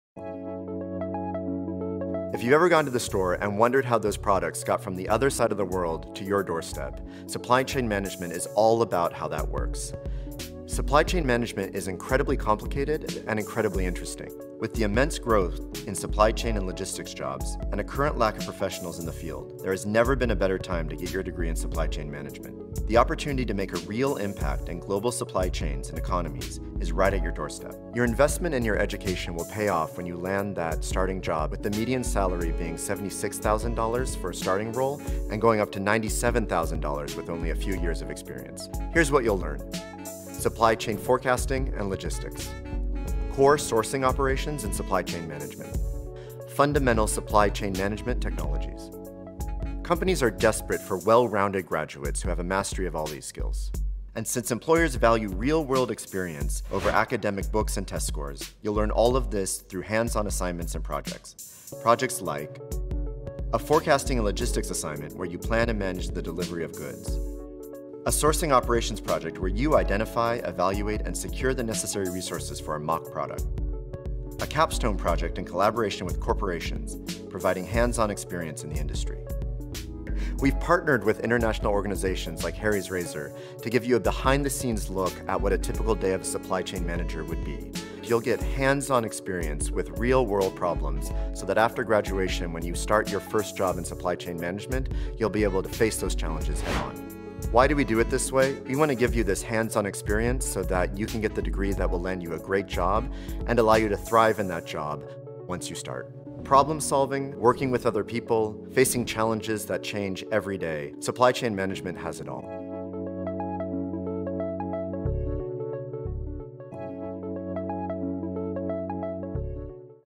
An Overview of the Supply Chain Management Major | Greensboro College
As a Supply Chain Management major at Greensboro College, you will learn how to use forecasting techniques to match supply and demand, and how to develop logistics networks that help minimize costs and deliver top customer service. Students will build experience in solving real-world supply chain problems and seeing how their solutions compare to actual professionals in the field.

Learn more about the Supply Chain Management major and program by visiting